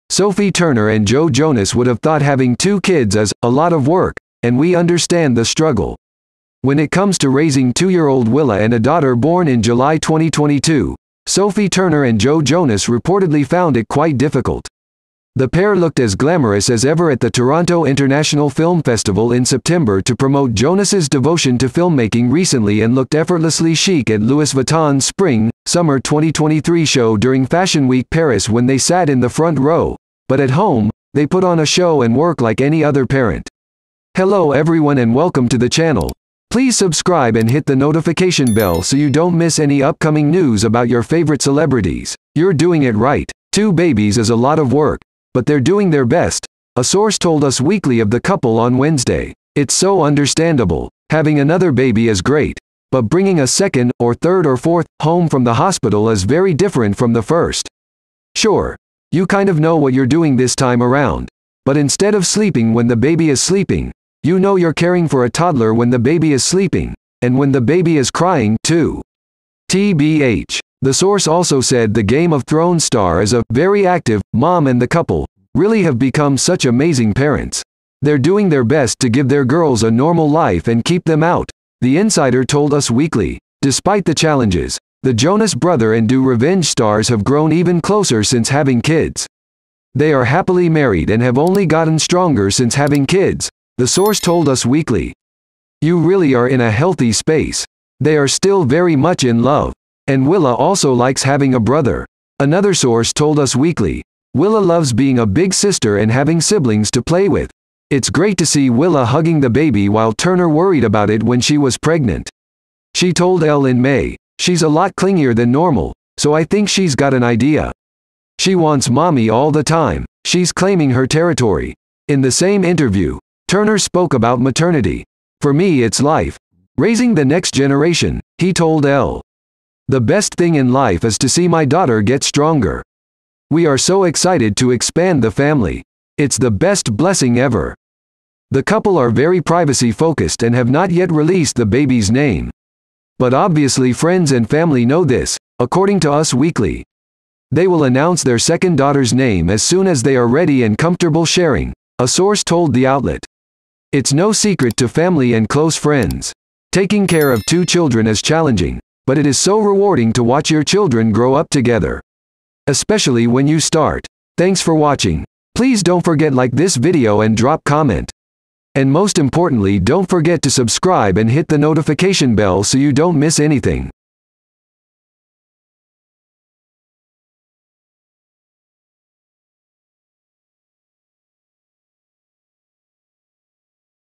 Sophie Turner and Joe Jonas talk on how difficult it is to take care of 2 kids
Sophie Turner and Joe Jonas would have thought having two kids is "a lot of work" and we understand the struggle! When it comes to raising 2-year-old Willa and a daughter born in July 2022, Sophie Turner and Joe Jonas reportedly found it quite difficult. The pair looked as glamorous as ever at the Toronto International Film Festival in September to promote Jonas' devotion to filmmaking recently and looked effortlessly chic at Louis Vuitton's Spring/Summer 2023 show during Fashion Week Paris when they sat in the front row, but at home, they put on a show and work like any other parent.
"You're doing it right. Two babies is a lot of work, but they're doing their best," a source told Us Weekly of the couple on Wednesday.
It's so understandable. Having another baby is great, but bringing a second (or third or fourth) home from the hospital is very different from the first. Sure, you kind of know what you're doing this time around, but instead of sleeping when the baby is sleeping, you know you're caring for a toddler when the baby is sleeping - and when the baby is crying, too. , TBH. The source also said the Game of Thrones star is a "very active" mom and the couple "really have become such amazing parents."

"They're doing their best to give their girls a normal life and keep them out," the insider told Us Weekly.
Despite the challenges, the Jonas Brother and Do Revenge stars have grown even closer since having kids. "They are happily married and have only gotten stronger since having kids," the source told Us Weekly. “You really are in a healthy space. They are still very much in love. And Willa also likes having a brother. Another source told Us Weekly, "Willa loves being a big sister and having siblings to play with."

It's great to see Willa hugging the baby while Turner worried about it when she was pregnant. She told Elle in May, "She's a lot clingier than normal, so I think she's got an idea. She wants mommy all the time — she's claiming her territory." In the same interview, Turner spoke about maternity. "For me it's life: raising the next generation," he told Elle. “The best thing in life is to see my daughter get stronger. We are so excited to expand the family. It's the best blessing ever. "

The couple are very privacy focused and have not yet released the baby's name. But obviously friends and family know this, according to Us Weekly. "They will announce their second daughter's name as soon as they are ready and comfortable sharing," a source told the outlet. "It's no secret to family and close friends."

Taking care of two children is challenging, but it is so rewarding to watch your children grow up together. Especially when you start